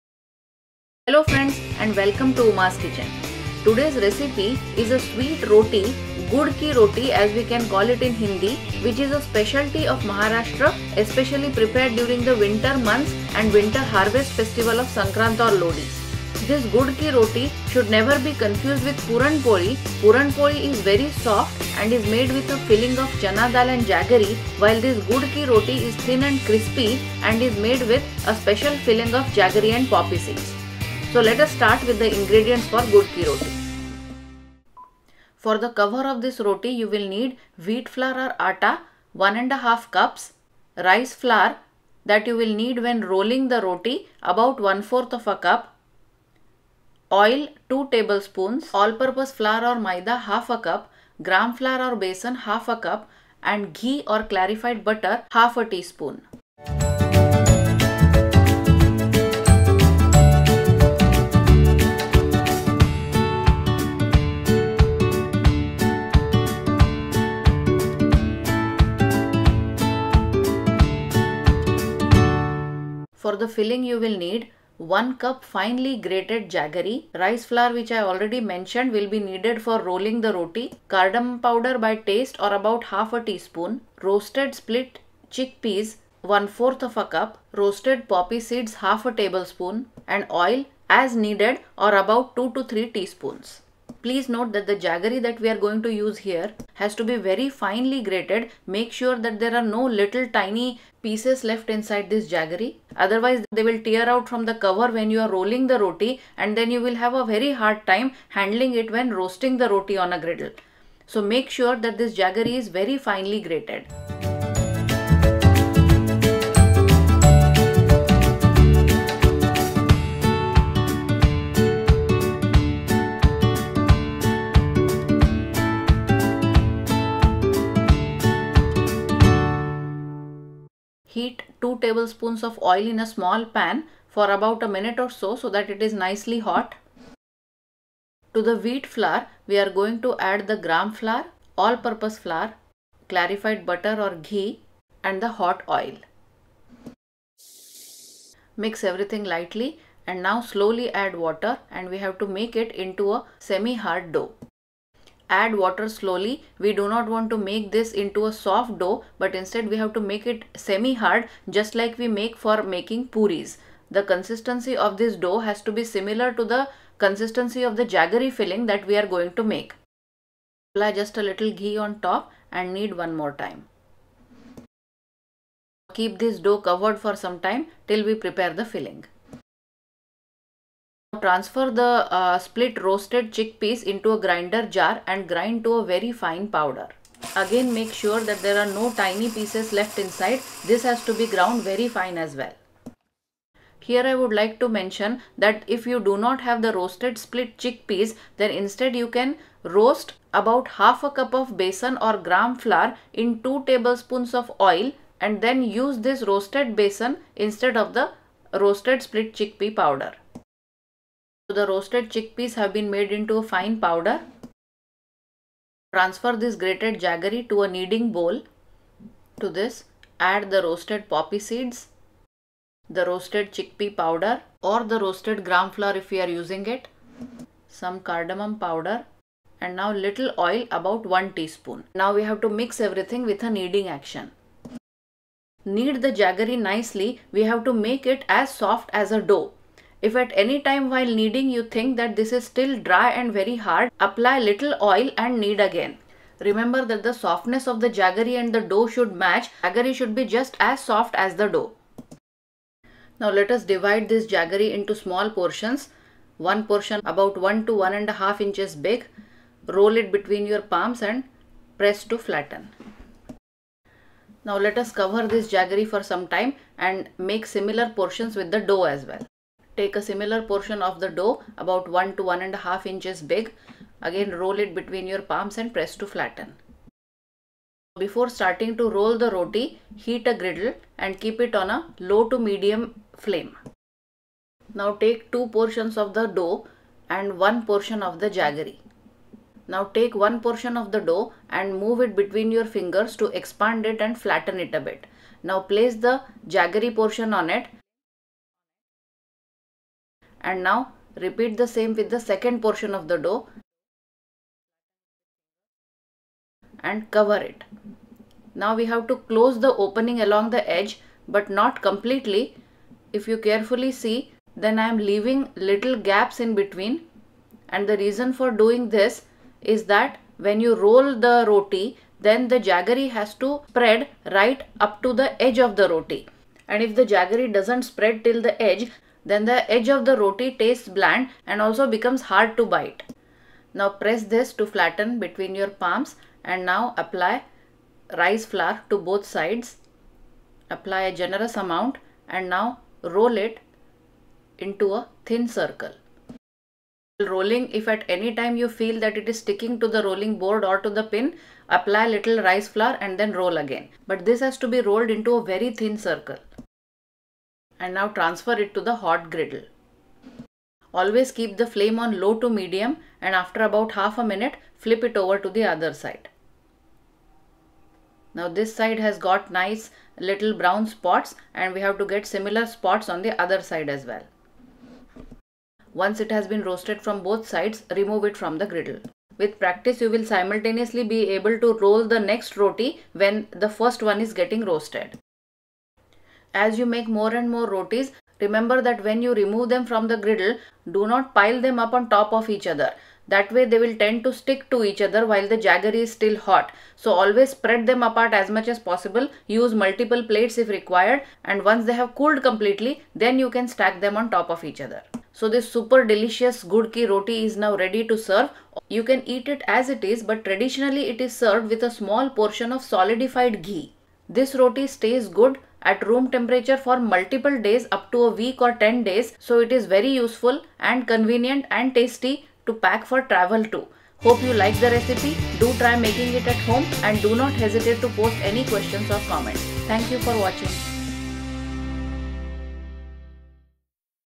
Gud Ki Roti Recipe-A Crispy Sweet Roti Made With Wheat Flour And Jaggery | Uma's Kitchen
Ingredients:

For the cover-

Wheat flour (Ata) - 1 & 1/2 cups
Clarified butter (Ghee) - 1/2 tsp
Rice flour - About 1/4 cup for dusting

For the filling:

Jaggery (gud) - 1 cup grated
Poppy seeds - 1/2 tbsp roasted
Cardamom powder - 1/2 tsp
Roasted split chick peas (Dalia) - 1/4 cup
Oil - As needed about 2-3 tsp

Instructions:
For making the dough :

Mix wheat flour, all purpose flour, and gram flour (besan).
Heat 2 tbsp hot oil and add to the flours in step 1.
Also add ghee (clarified butter) to it.
Add water to knead into dough, not very soft but not too dry as well (just like puri dough).

For the jaggery filling :

Add the roasted poppy seeds and cardamom powder to the grated jaggery.
Grind the dalia to a fine powder and add to the jaggery. (Instead of dalia you can also add 1/2 cup besan roasted in 2 tbsp oil. Roast till you get a golden brown color. Then add to the jaggery).
Knead the jaggery to make it soft, like the dough.
You can use little oil to knead, to make it softer.

Procedure :

Divide the dough into 1" - 1 & 1/2" parts. Roll each part between your palms, flatten, and keep aside.
Divide the filling into parts that are the same size as the dough parts. Just like the dough, roll them, flatten and keep aside.
Place one jaggery ball between 2 balls of dough and close the edge leaving little gaps in between.
Use rice flour to dust both sides and roll into a thin circle (about a mm thick).
Transfer to a hot griddle.
Roast both sides on low - medium heat, till a few dark brown spots appear.
Repeat for all the jaggery and dough balls.
Spread all the polis to cool, before piling them up for storing.
Always serve this delicious gud ki roti with some solidified ghee.